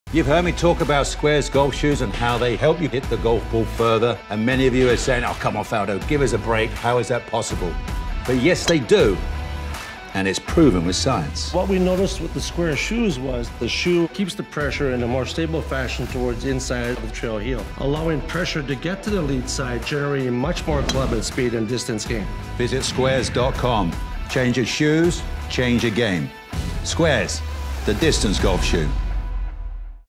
Introducing SPEED™ Golf Shoes from SQAIRZ!
SQAIRZ officially launches their SPEED™ Golf Shoes on March 31, 2021 SQAIRZ are the only golf shoes scientifically proven to generate more club head speed and distance! Change your shoes; change your game with SQAIRZ, The Distance Golf Shoe™.